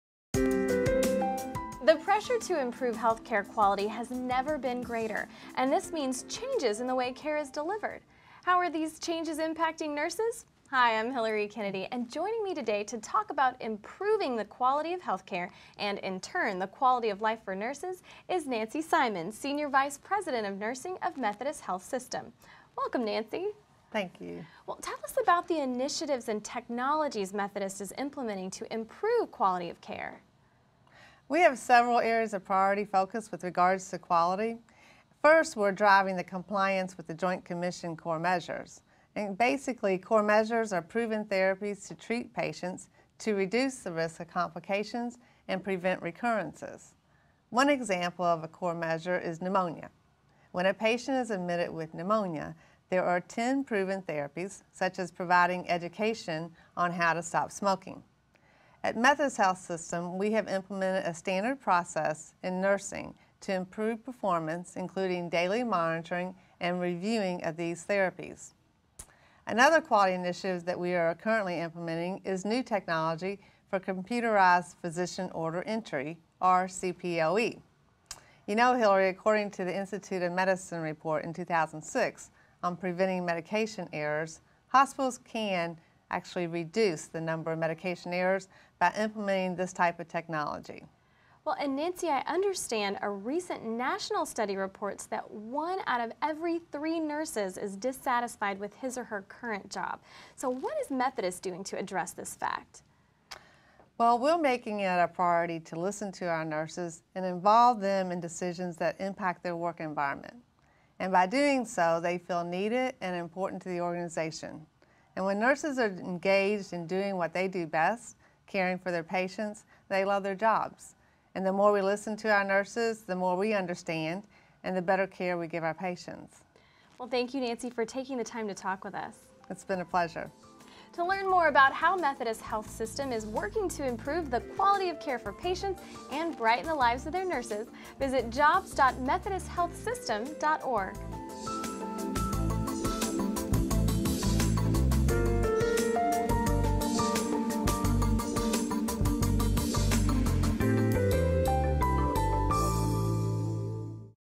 Methodist Health System: Improving Health Care and Brightening the Lives of Nurses
Nancy E. Simon, RN, BSN, MA, FACHE, Senior Vice President and Chief Nursing Officer at Methodist Health System, one of North Texas' most distinguished, nonprofit health care systems, discusses the system's initiatives to improve the quality of health care for patients and to brighten the lives of its nurses.